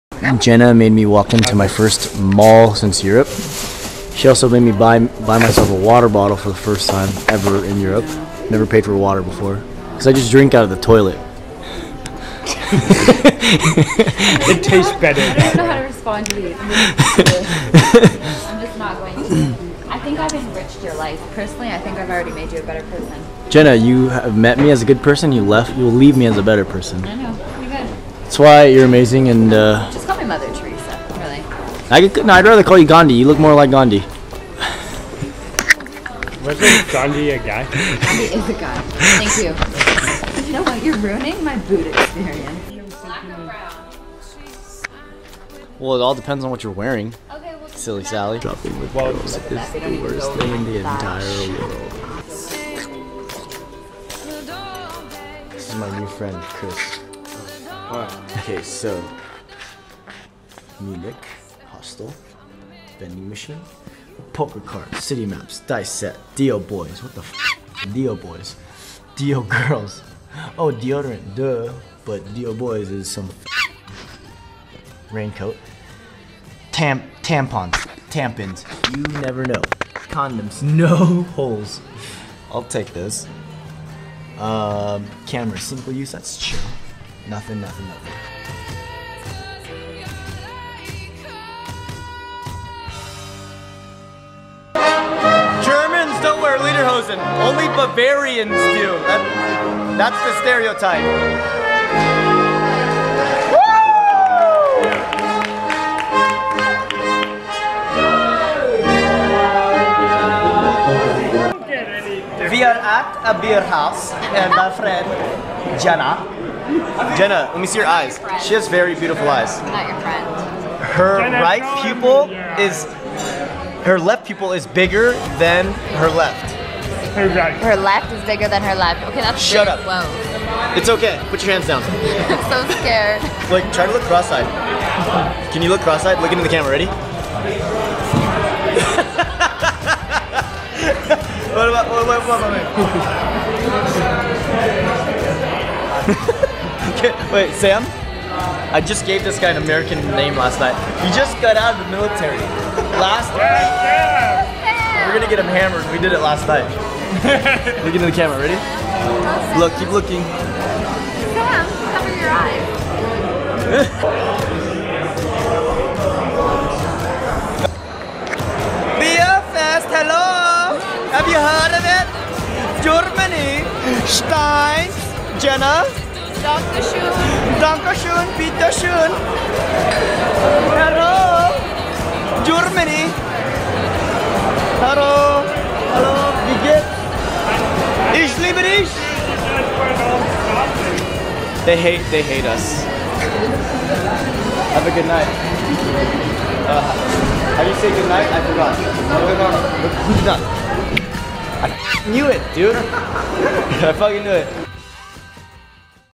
Munich Part 2 Vlog #29
My first last night in Munich, I say first because I know I'll be back here soon! I get the feeling of Seoul here. It is definitely a little more conservative then most places that I visited. Things are a bit pricey but it is worth every penny!
I got to make many new friends coming from all sorts of backgrounds. It's good to mix cultures and learn new things about strangers who then turn into friends.  I am truly blessed be able to travel around like this feeling free from everything.

Munich, is full of history, starting from the bavarians to hitler.  Hof Brau Haus is a major tourist spot, and I usually don't do touristy stuff toooo much, but I highly recommend having a meal and at least a few beers here, as i did myself.
For those of you girls, shopping would be a major hit here as well..!

If you have any questions about my trip, just ask!